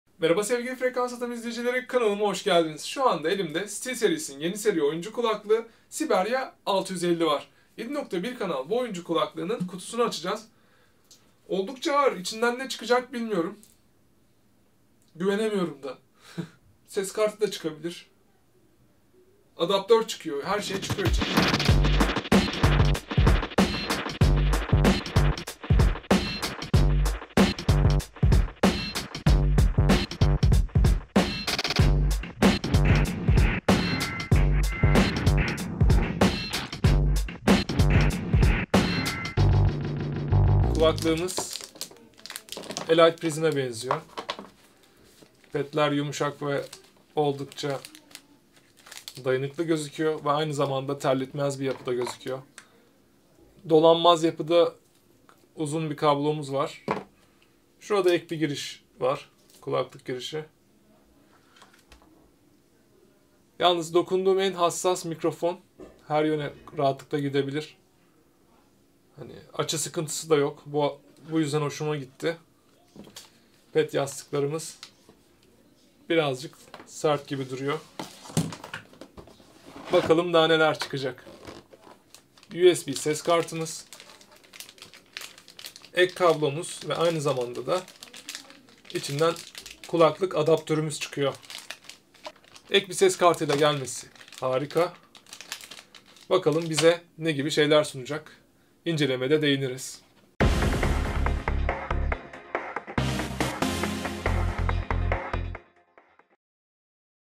Steelseries Siberia 650 7.1 Dolby Oyuncu Kulaklığı ''Unboxing''
Steelseries Siberia 650 7.1 Dolby Oyuncu Kulaklığı ''Unboxing''''Unboxing''

 Steelseries'in 7.1 kanal oyuncu kulaklığı 650 kutusundan çıkıyor. Harici ses kartı ile gelen kulaklık oyun severler için ek ses paylaşım portuna sahip. Hem oyun oynarken hem de arkadaşınızla ortak vakit geçirirken eğlenebileceğiniz bu kulaklık Elite Prism kullanan arkadaşların da aşina olabileceği bir tasarıma sahip.